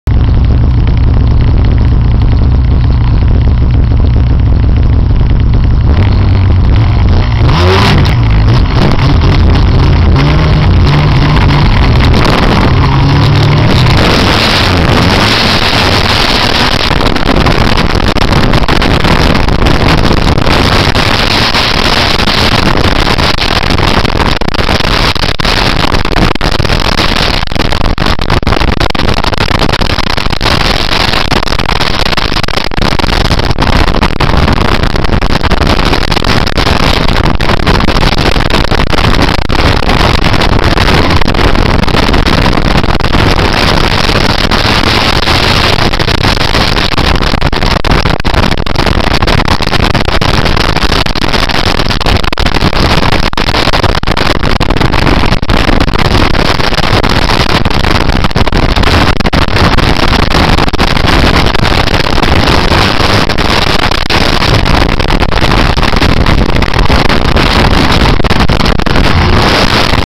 MICHIGAN UP, Hood CAM. Two F7 "K-9's"
Snowmobiling down winding trail in the U.P. at 70 MPH.  Hood Mounted cam catches all the action.